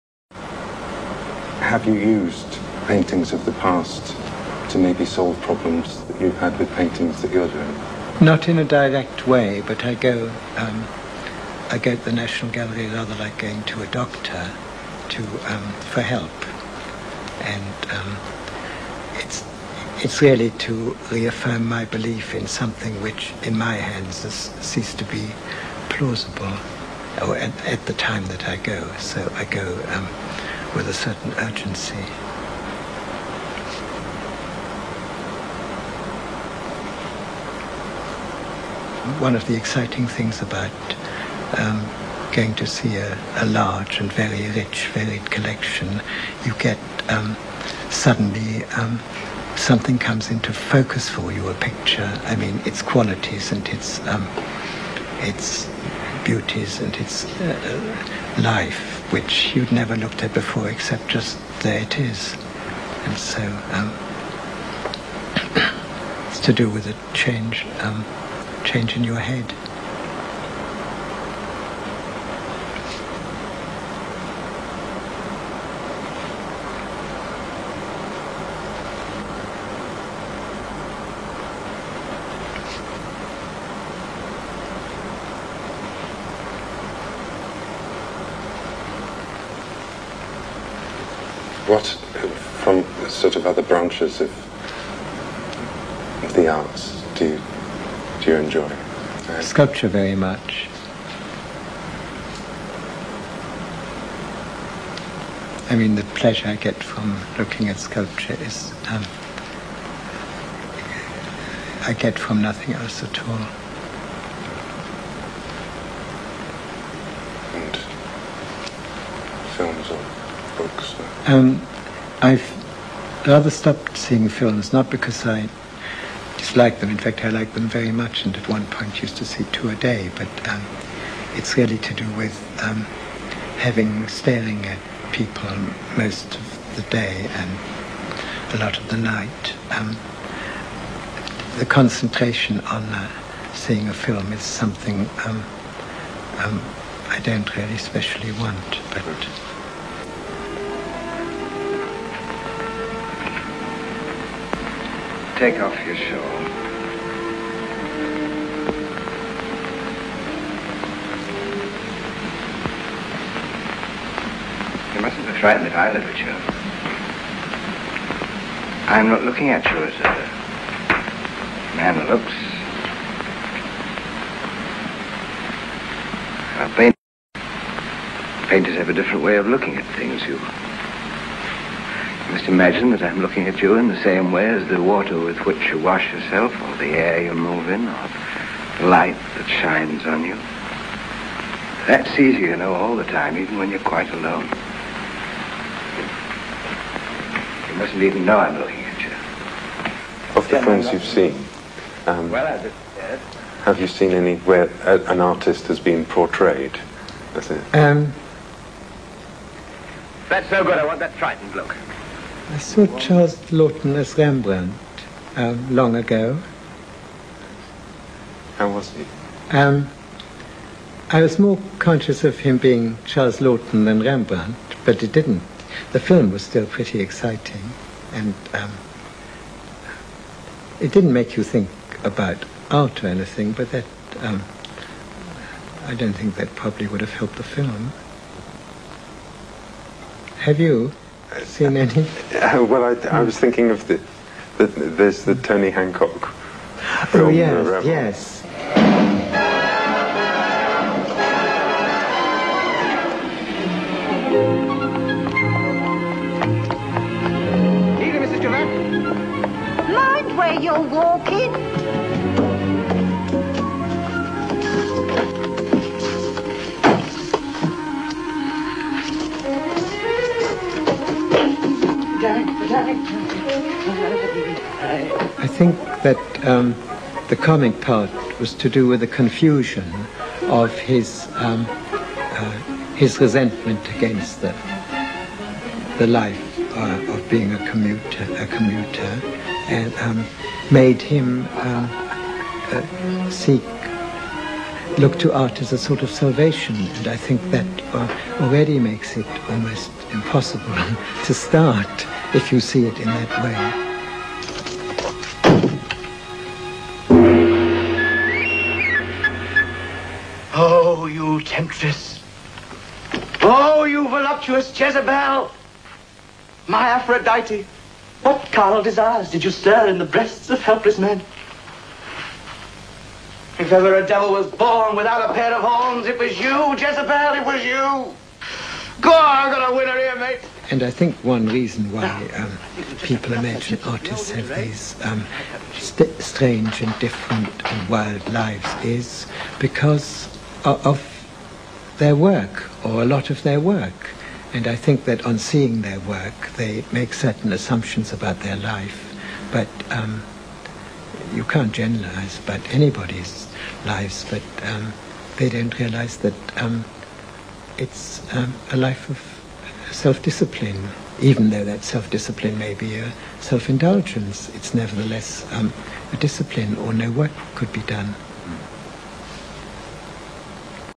Lucian Freud's RAREST Interview - Part 4/5
The RAREST interview of the pre-eminent British artist of his time, Lucian Freud.
Found NOWHERE on the web, except here, on Art-Sheep Tv!